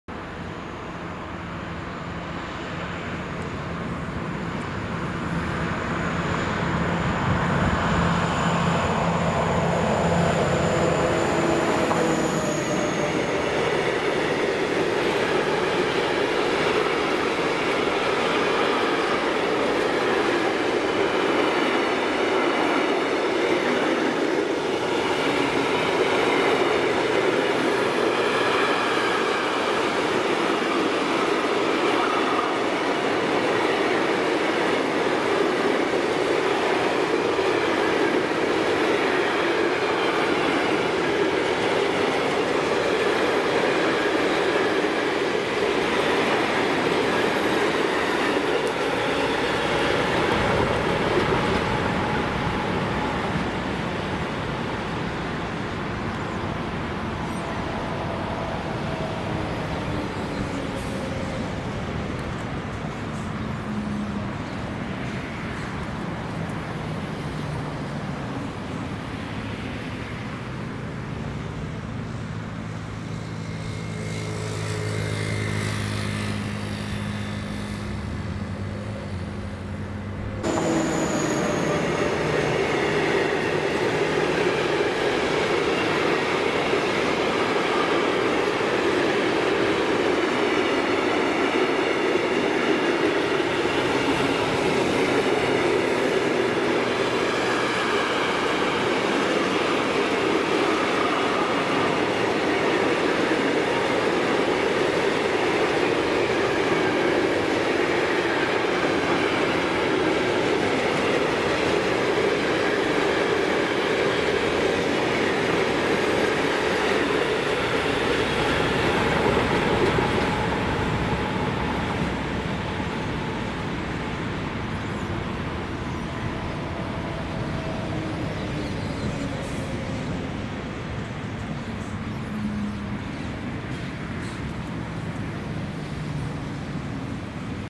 RTB Cargo G1206 V159(Diesel) at Rotterdam #1 , Oct/2018 RTB Cargo G1206 V159ディーゼル機関車
RTB CARGO GmbH, based on Düren, Germany. This is Diesel locomotive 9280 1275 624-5 D-DISPO built by Vossloh(Werdohl, Germany). This is Harbour called Waalhaven near Europort in Rotterdam.
ドイツのデュレンを本拠地とするRTB カーゴのディーゼル機関車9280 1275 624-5 D-DISPOは、ドイツのWerdohlを本拠地とするVossloh社によって製造されました。ここはオランダのユーロポート近くのワールハーヴェン港です。
RTB es una empresa de Logística y Transporte cuya sede está en Düren, Alemania. Esta locomotora Diesel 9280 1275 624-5 D-DISPO fue fabricada por Vossloh(Werdohl, Alemania). Este lugar se llama Waalhaven cerca de Euro-Puerto en Rotterdam.

Rotterdam, the Netherlands, October 2018